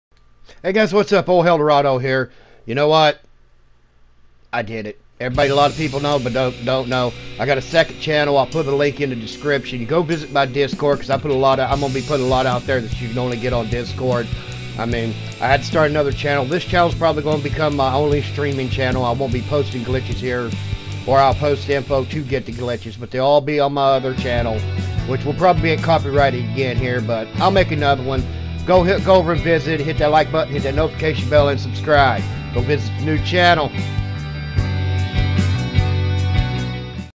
NEW CHANNEL CHECK IT OUT
🤑IN THIS VIDEO I WILL BE SHOWING A FAST MONEY AND XP METHOD !!!🤑

PLEASE 24 HR MEANS ONLY WORKING FOR 24HRS THAN CHANGES AT 8PM EST!!!PLEASE USE THE COLLECTORS MAP BELOW!!
COLLECTABLES PRICE LIST